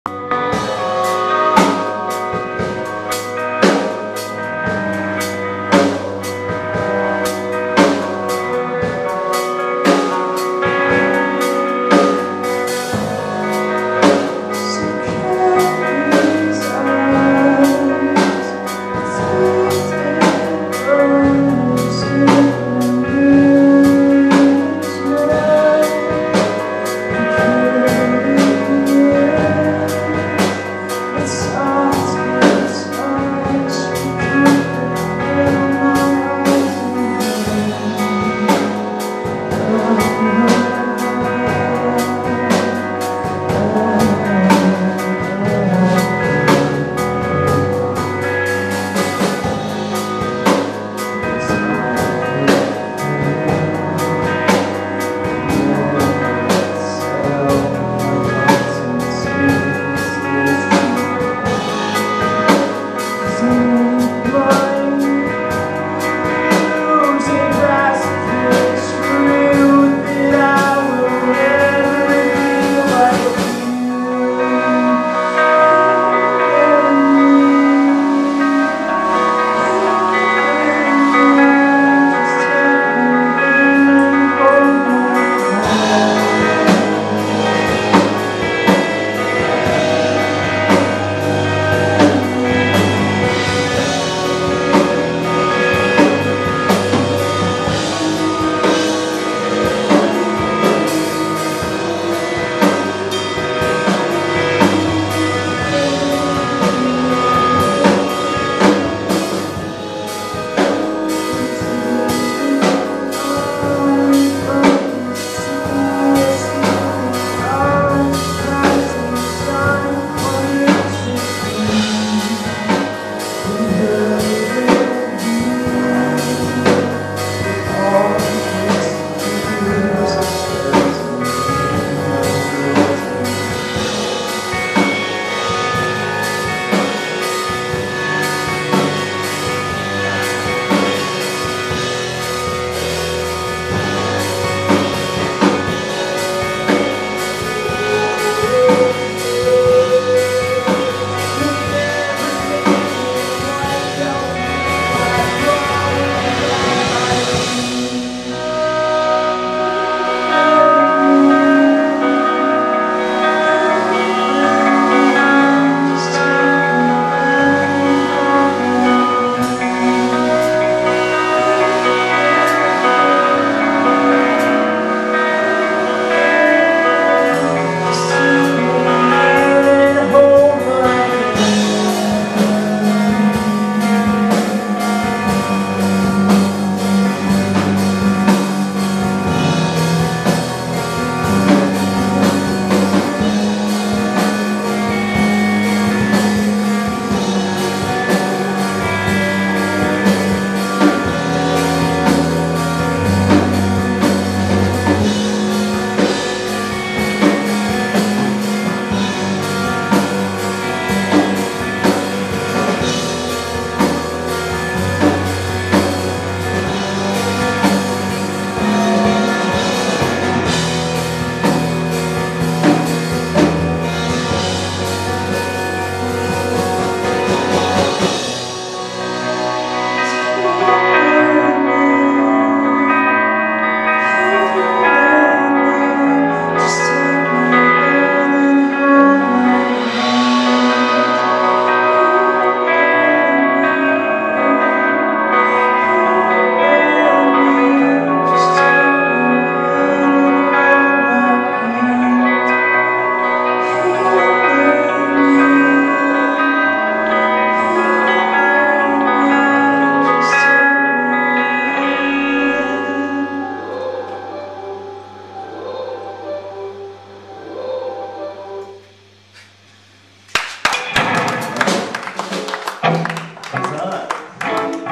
How to Operate Your Brain performs at Veracity
How to Operate Your Brain performs at the May Veracity show at the Vera Project on 5-9-12